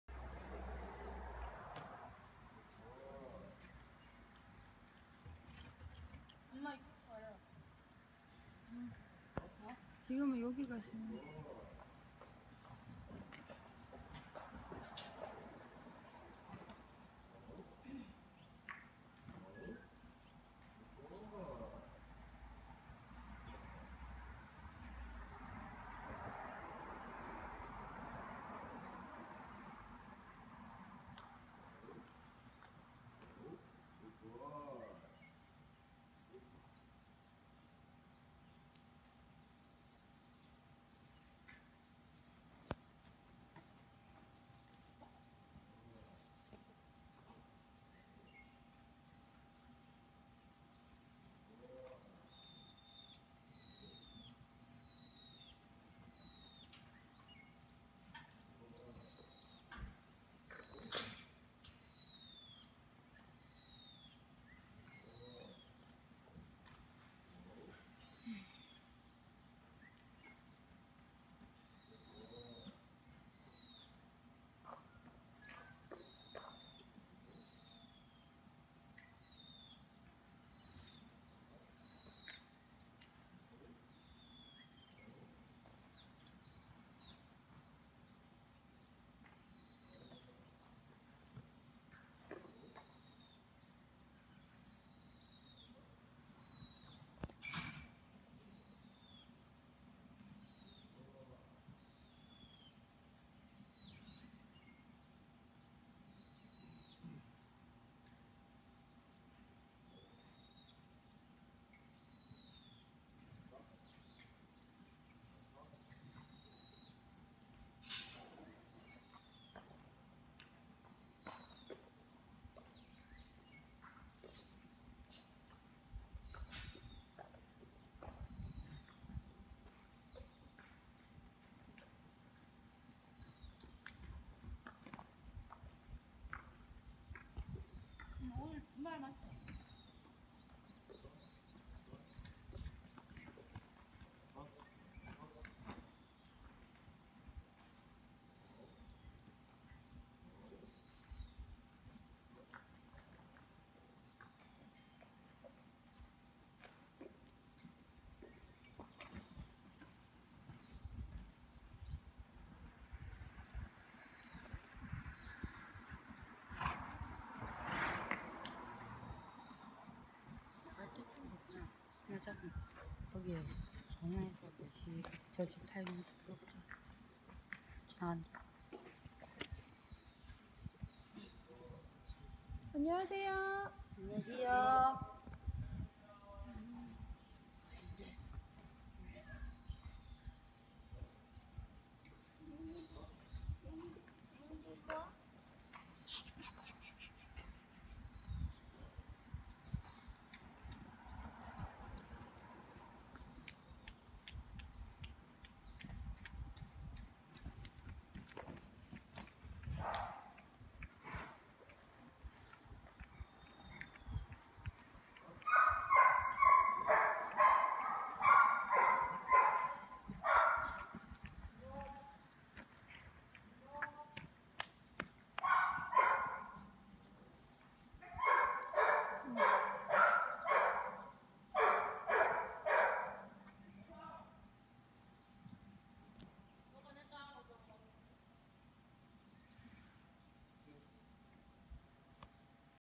Natural Horsemanship (자연인마술) 원형마장 없이 하는 서열 정립 : 세자리오
원형마장이 없을 때 조마삭으로 서열을 정립하며 사람에게 집중할 수 있는 방법입니다.